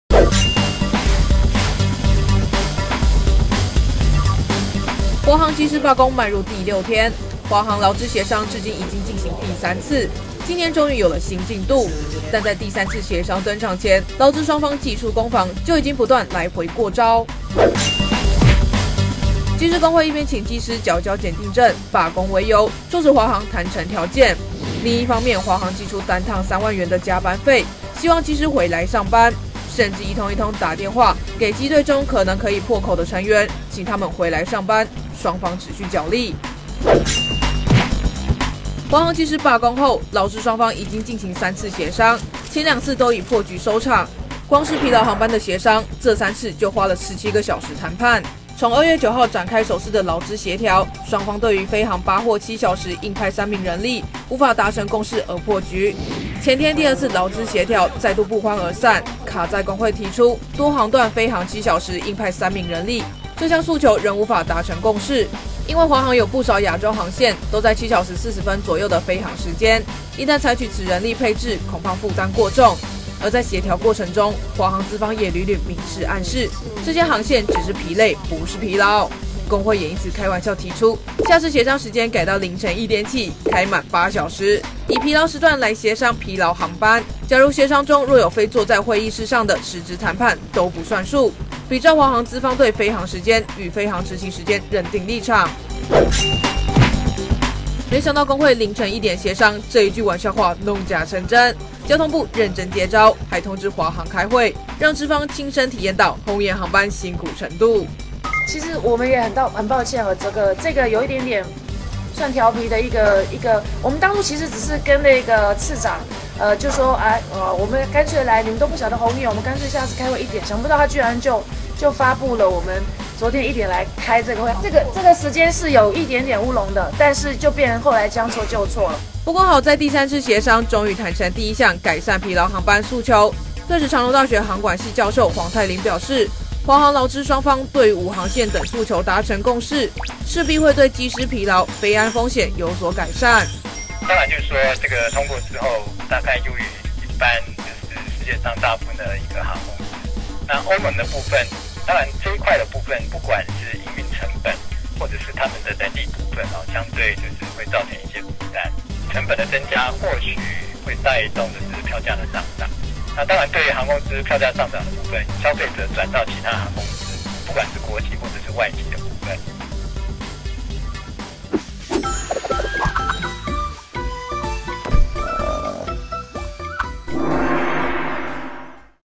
【華航談判】17小時馬拉松協商 勞資角力疲勞航班派遣 | 台灣蘋果日報
華航機師罷工邁入第6天，勞資三度協商，不過光第一項訴求改善「疲勞航班」就談了好久，前2次協商再加上今天凌晨的第3次協商，該項訴求就花了17個小時談判。今凌晨工會先釋善意，揚棄以往所提飛時7小時以上派遣3人納通案訴求，要求特殊航線逐一檢視...